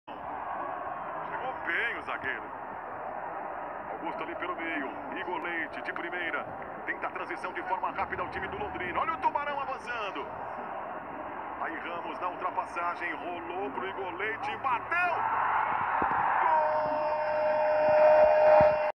Gol de Igor Leite - Londrina 1 x 0 Corinthians
Confira o gol de Igor Leite!

Londrina 1 x 0 Corinthians